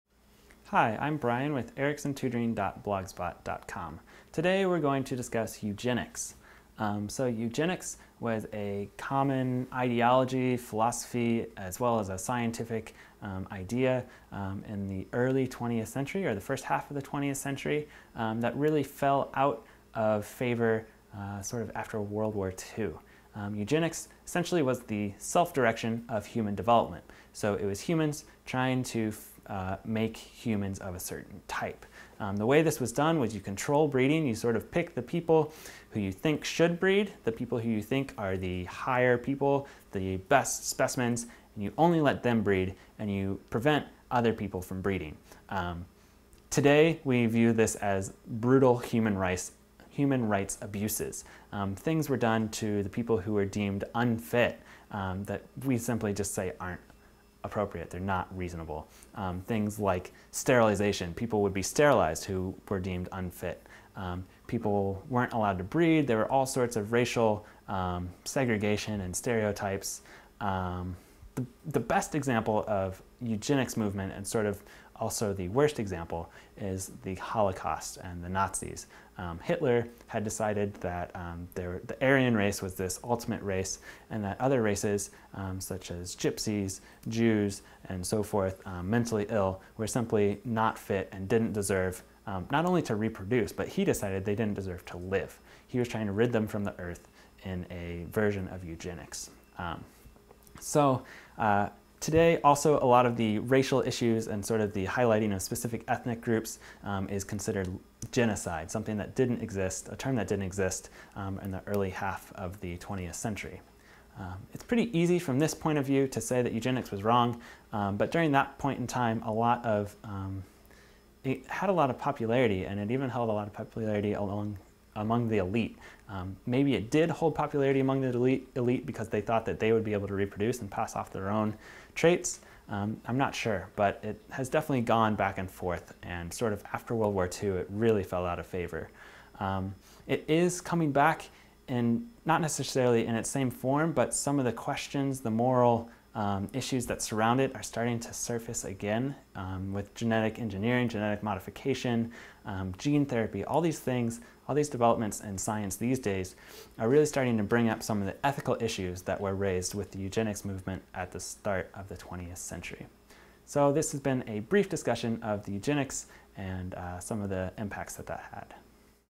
Biology & Chemistry : What Is Eugenics?
Eugenics was a common ideology or philosophy, as well as a scientific idea, that involved the self-direction of human development. Learn about the fallout of eugenics as a form of human rights abuse with help from a science teacher and field biologist in this free video on eugenics.